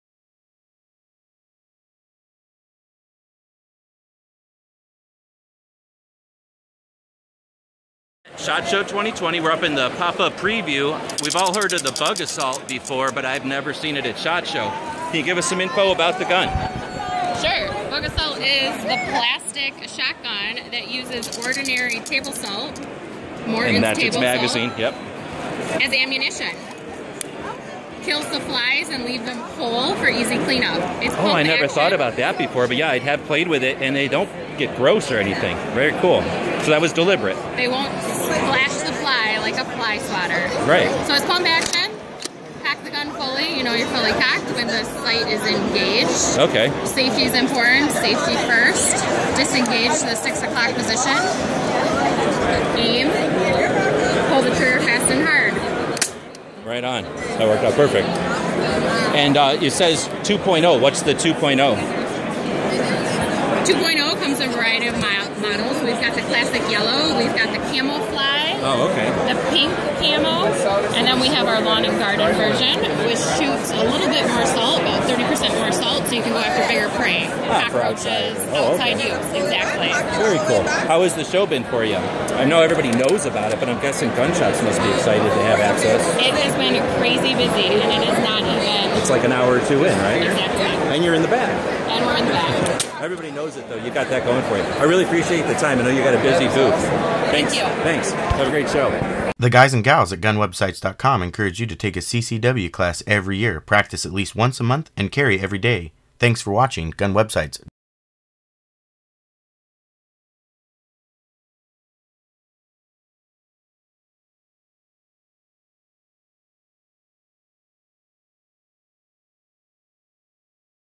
Bug a Salt at SHOT Show 2020
We visit SHOT Show each year (for the last 15 years) as part of our Gun Show Loophole Tour